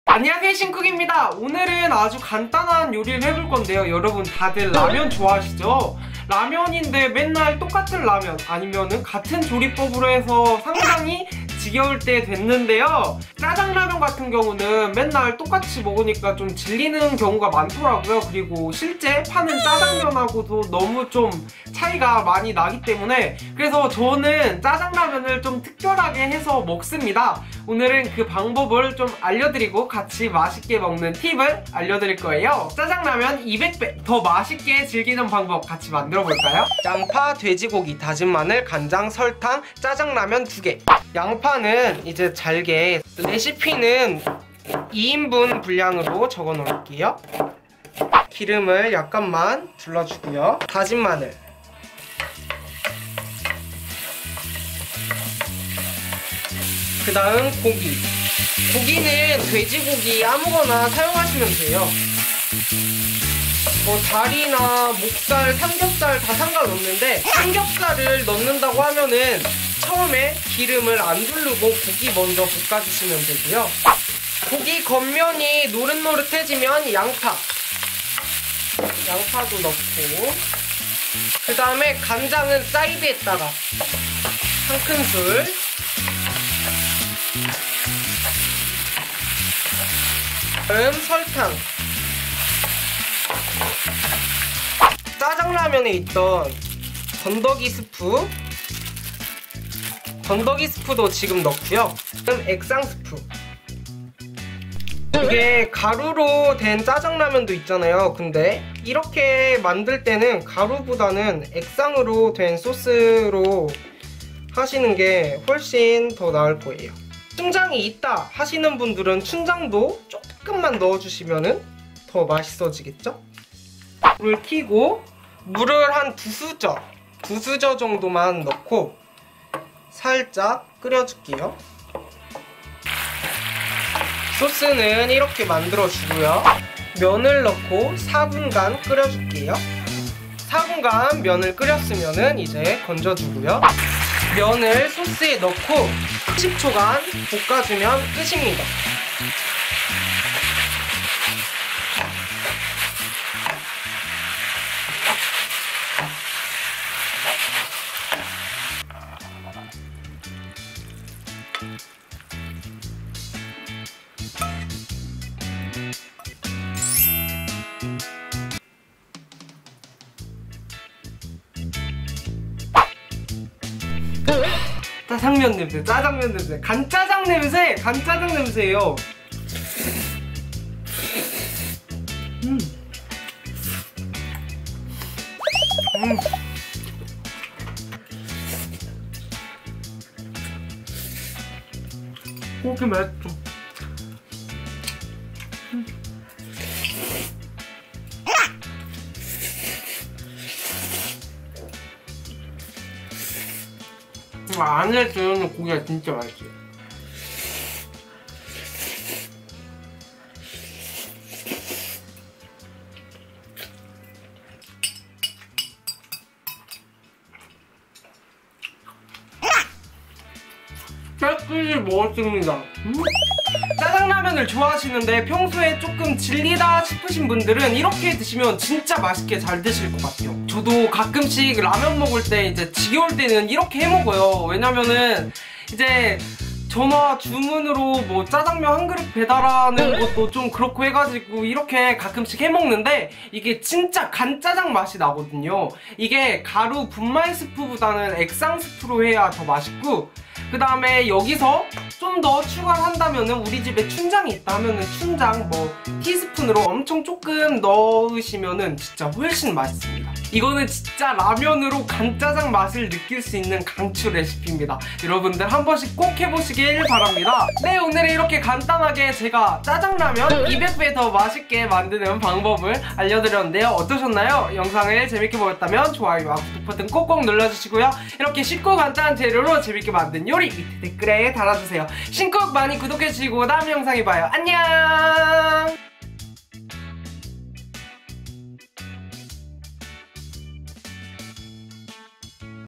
럭셔리 자취요리! 짜장라면으로 간짜장 만들기!! 짜장라면 200배 더 맛있게 먹는법!! SINCOOK-신쿡
짜장라면으로 간짜장 맛을 낼수있다!! 짜장라면 200배 더 맛있게 즐기는 꿀팁 입니다!!
저도 자주 해먹는 레시피에요 ㅎㅎ

-재료-
짜장라면 2개
양파반개
돼지고기 한줌
간장1T
설탕1T
물2T